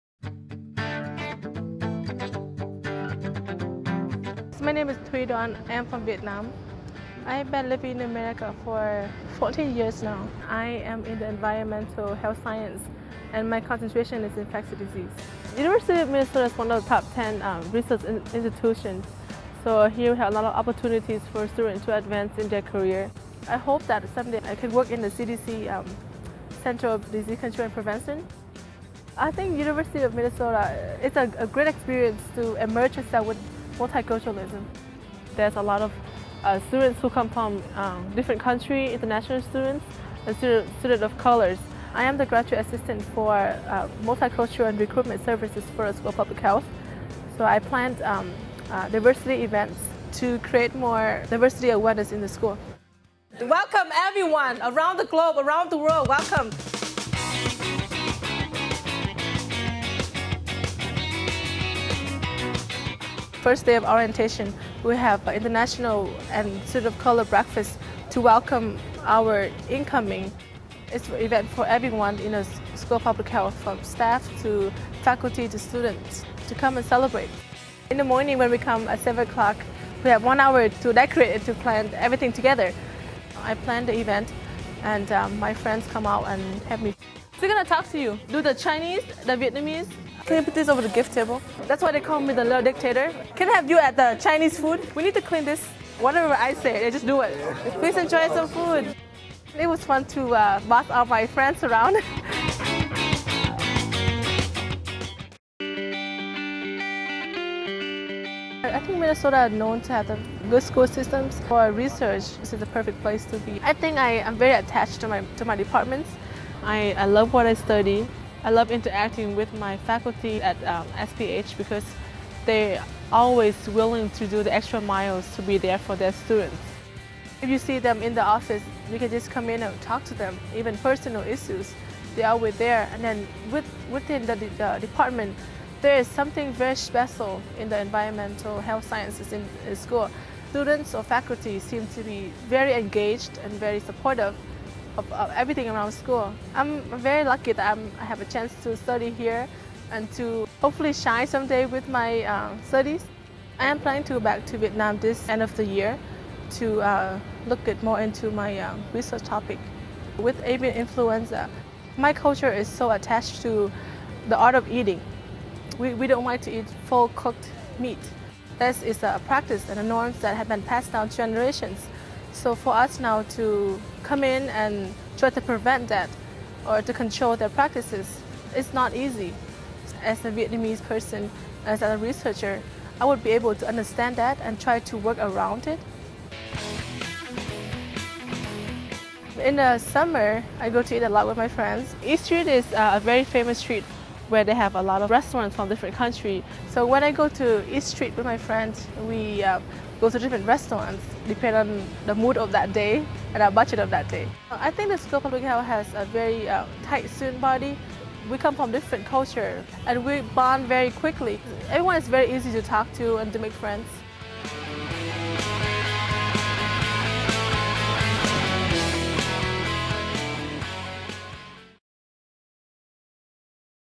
My Life: Thuy Doan
Thuy Doan, born in Vietnam, is seeking an MPH in Environmental Health Sciences, with an emphasis in infectious diseases. She plans to return to Vietnam next year to complete her field experience.

Thuy also works in multicultural recruitment services for the School of Public Health.